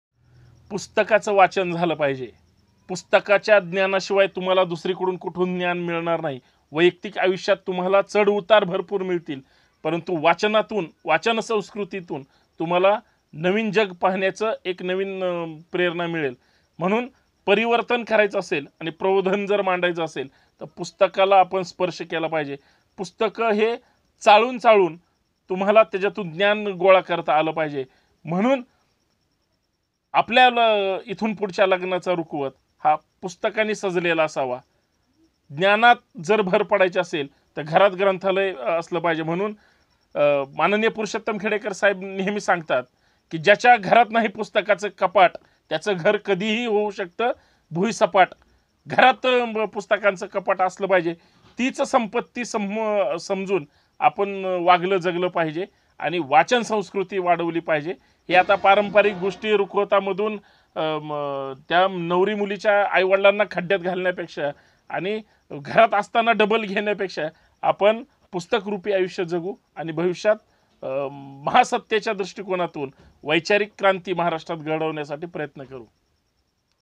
ज्याच्या घरात नाही पुस्तकाचं कपाट, त्याचं घर केव्हा ही होऊ शकते भुईसपाट | ब्रिगेडी | Santosh Shinde
मुलीच्या लग्नात खर्च कमी करून तो पैसा तिच्या नावाने FD किंवा पॉलिसीत गुंतवा... विचारधन म्हणून पुस्तकांचा रुकवत द्या...

- संतोष शिंदे, संभाजी ब्रिगेड, महाराष्ट्र